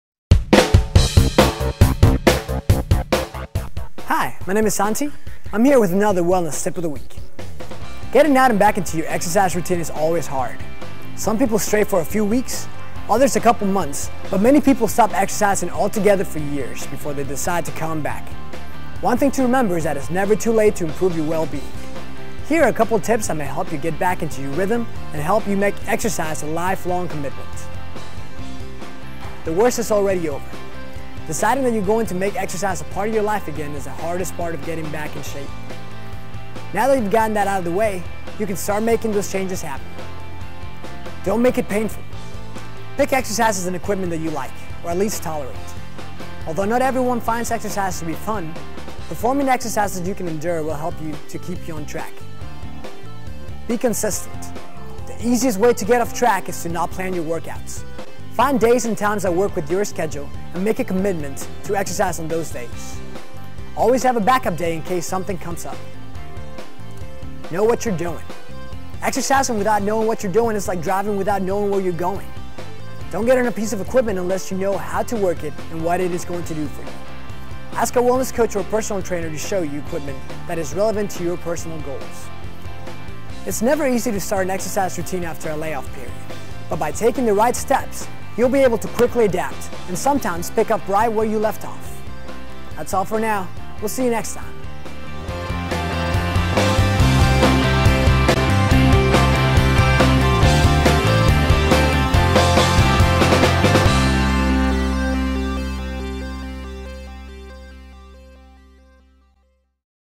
Getting Back in the Saddle
Many people find it tough to get going again if they've been away from a regular workout routine for some time.  In this week's tip, learn some tricks to make it easier to get back in the groove!